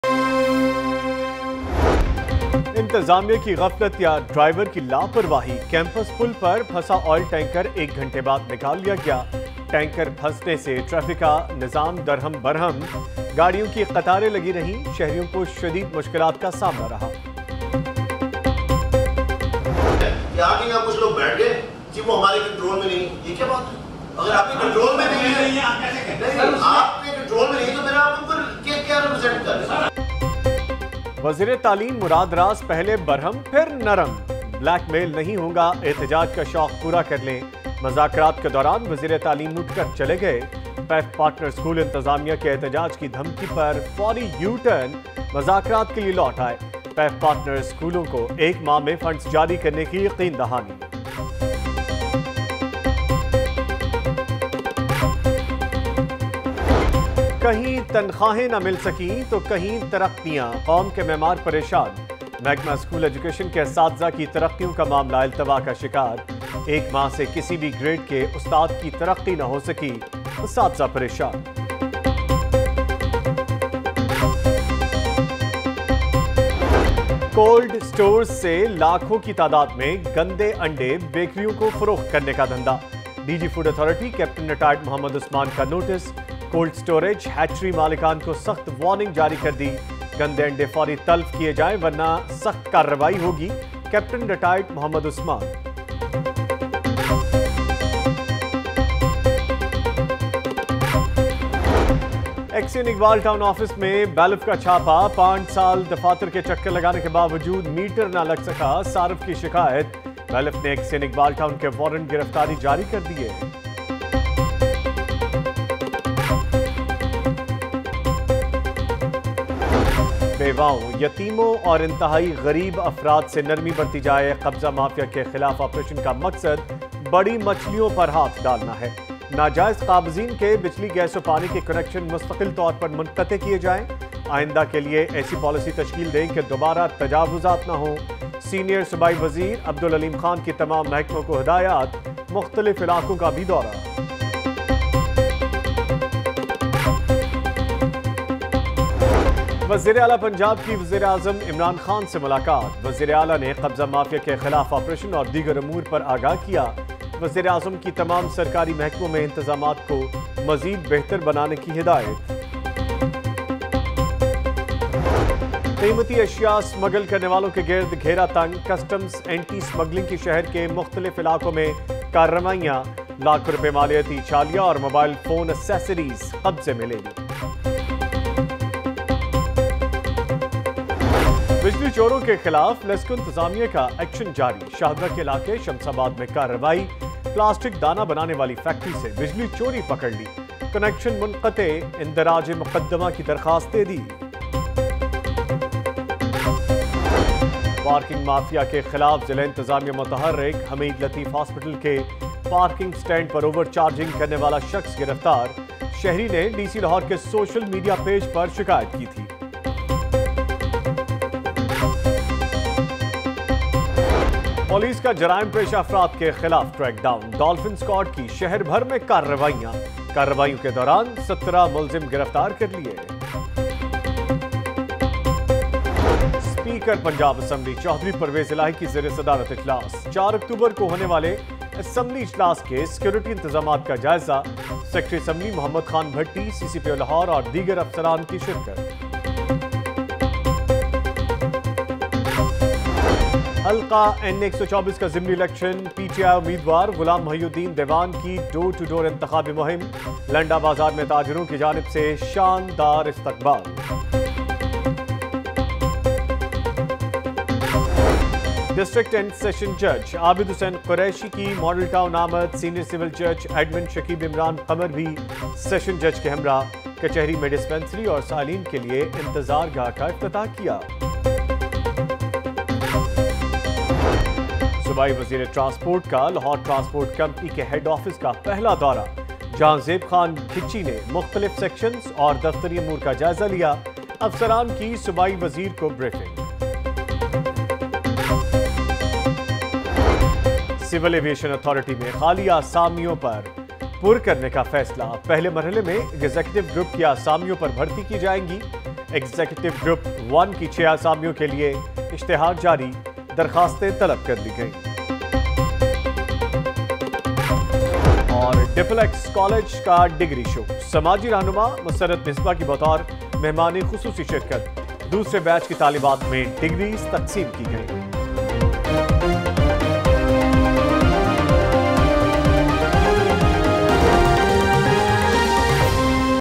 08 PM Headlines Lahore News HD - 01 October 2018
Watch Latest News : Pakistan Railways inaugurates non-stop train from Lahore to Faisalabad, Federal Minister Railways Sheikh Rasheed inaugurates non-stop train from Lahore to Faisalabad, Shahbaz Sharif's son-in-law Ali Imran to appear before court, Court rejects guardianship application of Yousaf Raza Gillani's daughter, Fawad Hassan Fawad to appear before court on 15th October in alleged corruption allegations against him.